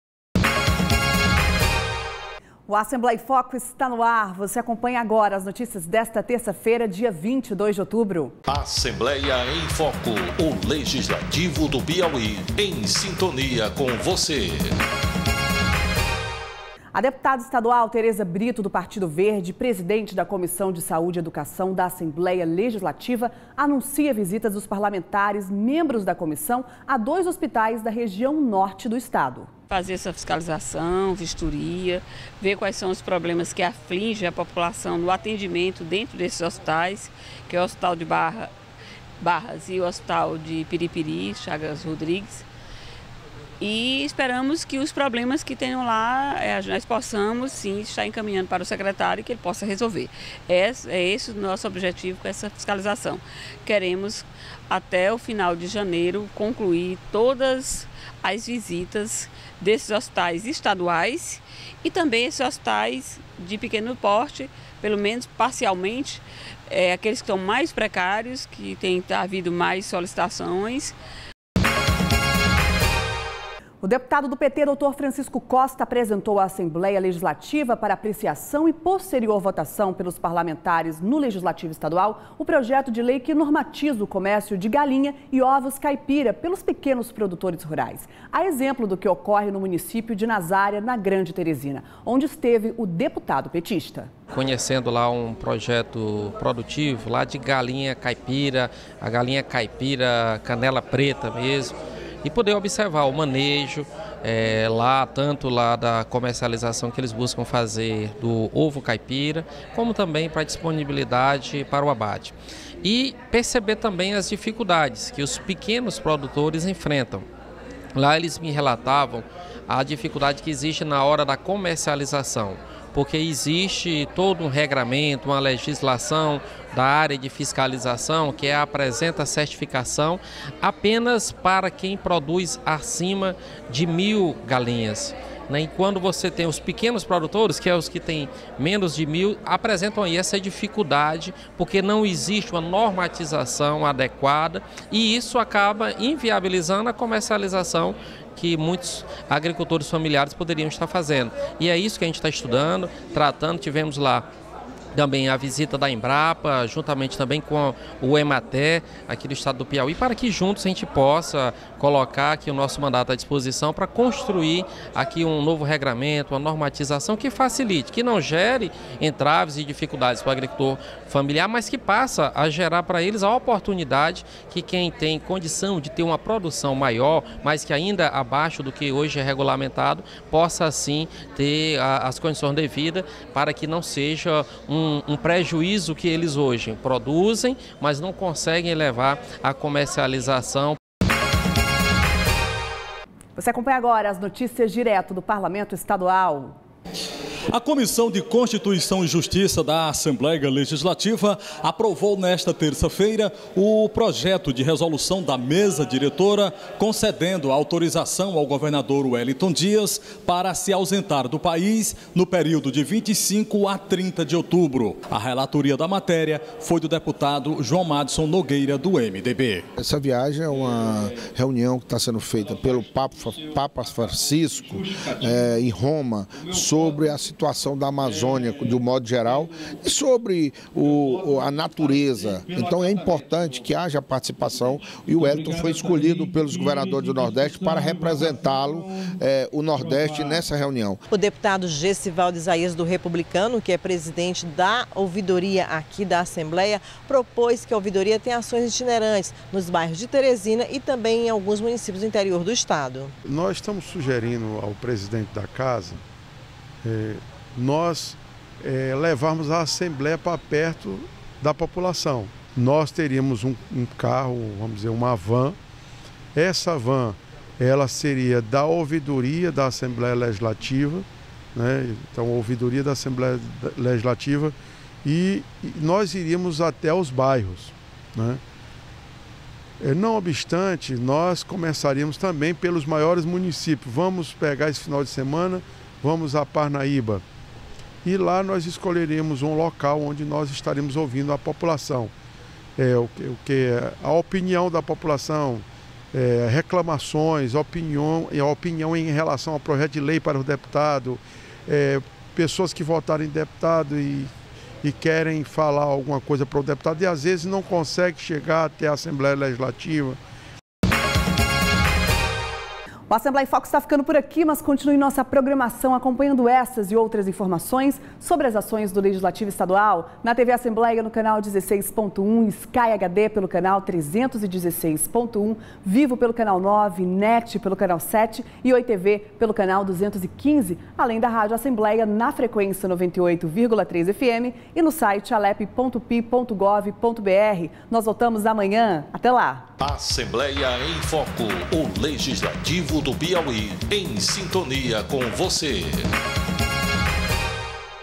Assembleia em Foco (22.10.19) - Projeto normatiza comercialização de pequenos criadores
Projeto de lei de autoria do deputado Dr. Francisco Costa, do PT, normatiza a comercialização de pequenos criadores de galinha e de ovos caipira. Deputada Teresa Britto, do PV, presidente da Comissão de Saúde da Assembleia, anunciou novas vistorias a hospitais do interior do estado. A CCJ aprovou autorização de viagem do governador Wellington Dias ao exterior. E o deputado Gessivaldo Isaías, do Republicanos, destacou importância da Ouvidoria Parlamentar Itinerante, proposta pelo parlamentar à Assembleia Legislativa.